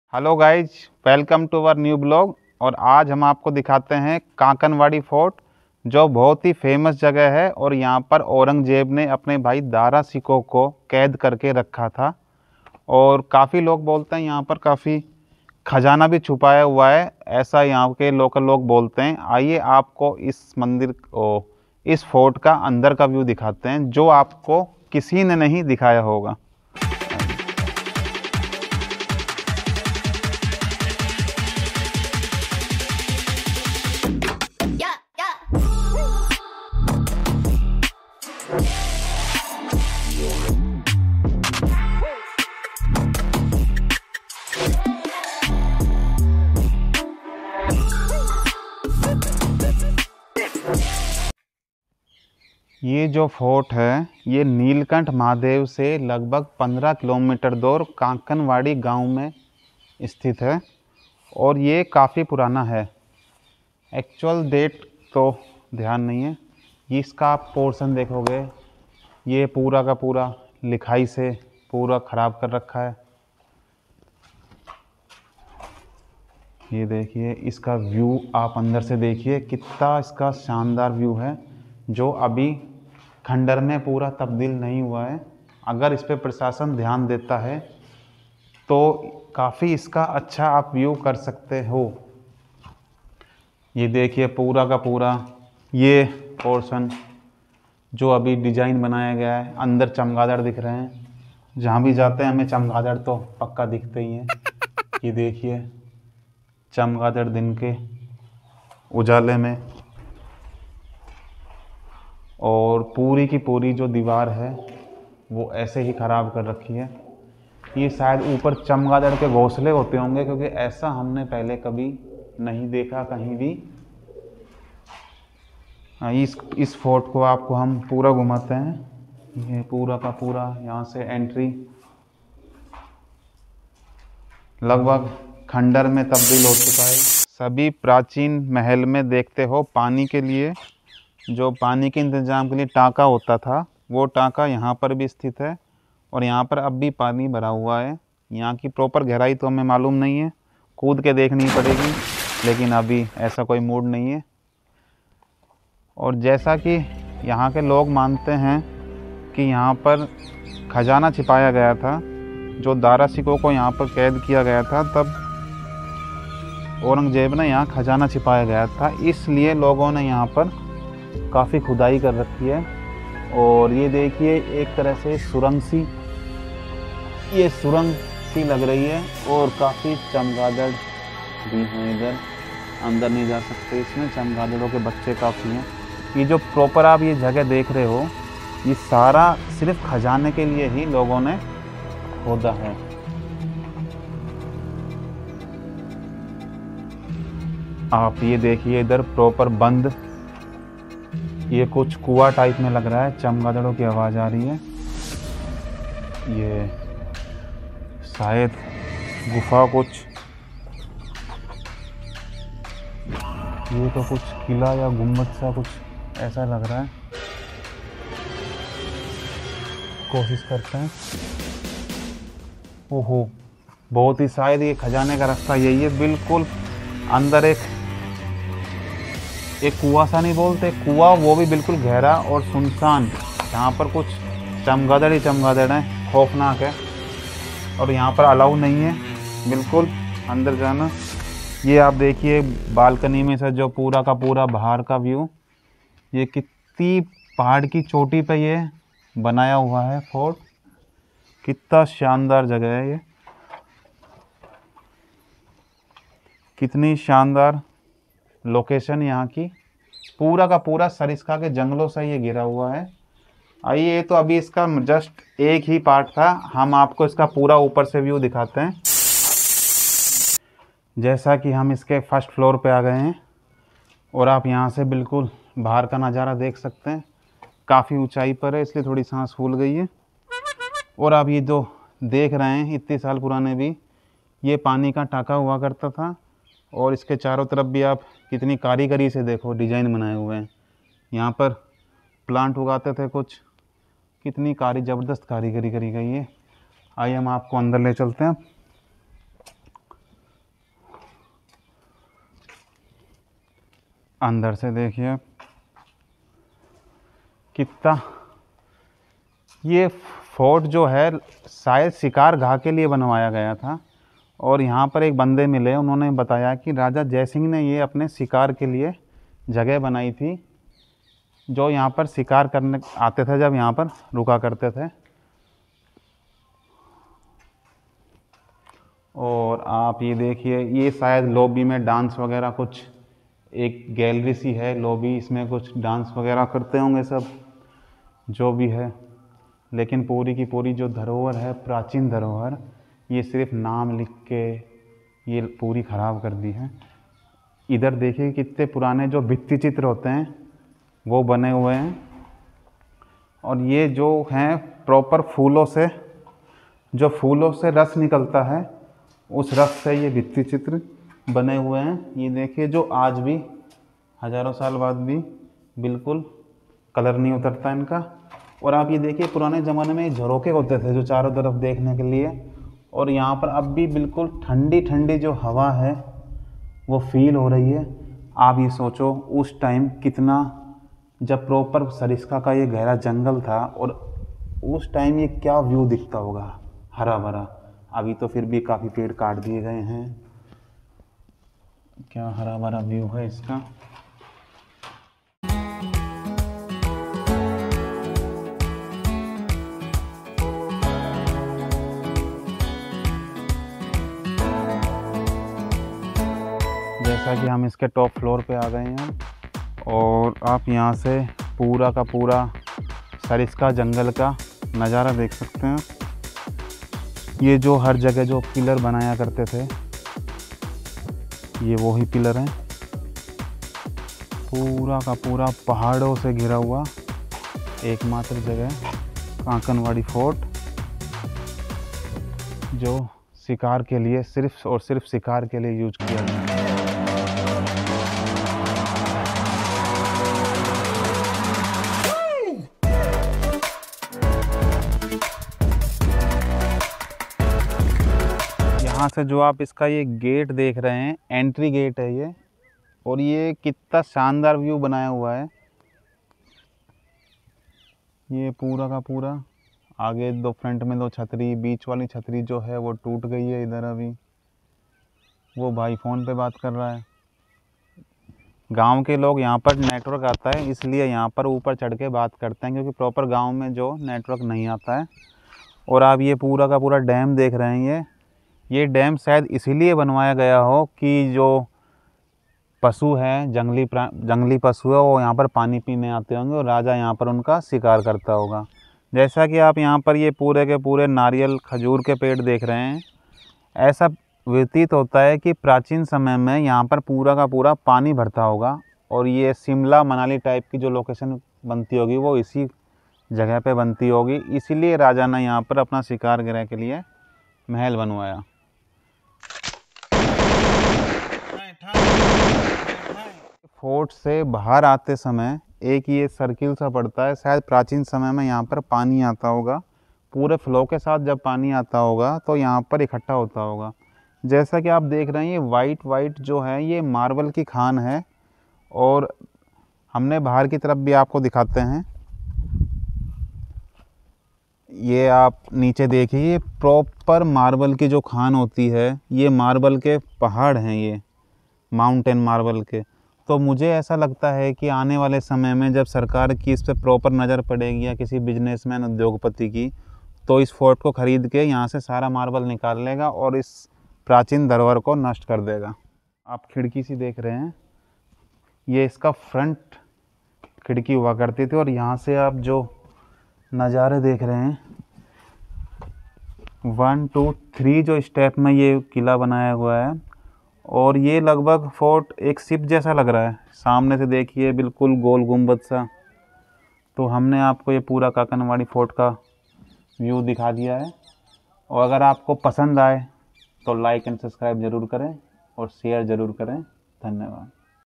Kankwari Fort || कांकवाडी किला
कांकवाडी किला राजस्थान के अलवर जिले मे स्थित है। यह  सरिस्का tiger पार्क के घने जंगलो के बीच स्थित है।
महाराजा जय सिंह ने इसे अकाल पड़ने पर बनवाया था ताकि लोगो को रोजगार मिल सके।
बाद मे औरंगजेब ने अपने भाई दाराशिकोह को यहा कैद कर के रखा था। और बाद मे उसका कत्ल कर दिया था
यह एक बहुत ही खूबसूरत किला है। यह नीलकंठ महादेव से 15 km आगे kankwari village मे पड़ता है।